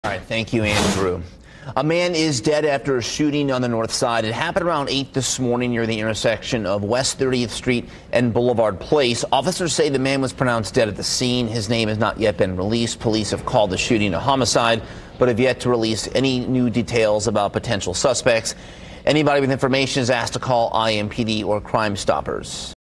Man killed in north side shooting
A man died early Sunday morning following a shooting on the city’s near north side, according to police.